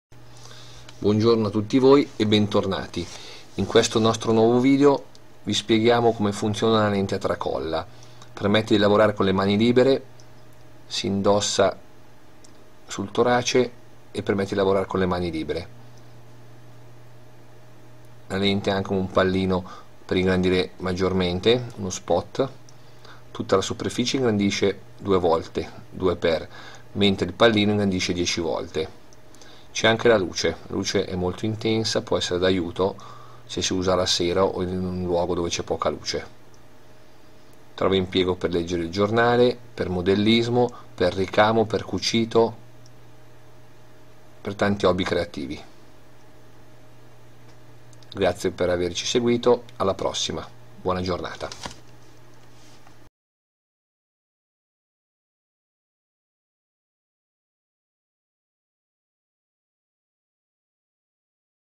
lente tracolla con luce. Lente per ricamo e per lavori precisi. Lente da lavoro
Lente tracolla con luce. Ottima per ricamo, modellismo e per leggere libri e giornali. Lente utile da usare in tante occasioni.